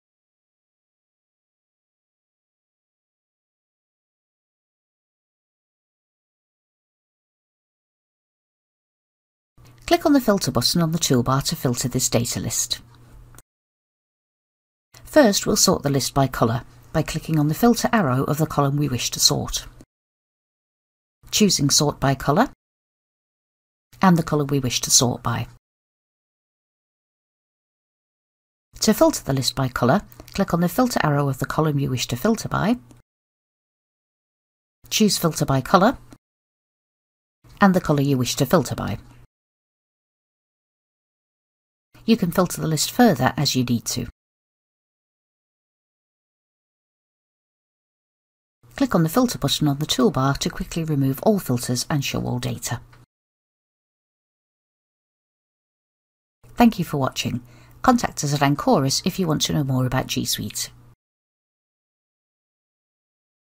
Sort and filter by colour in Google Sheets
It is now possible to sort and filter data by colour in Google Sheets. You can sort and filter data by both text and background colour, to make it easier to find relevant data in your sheets.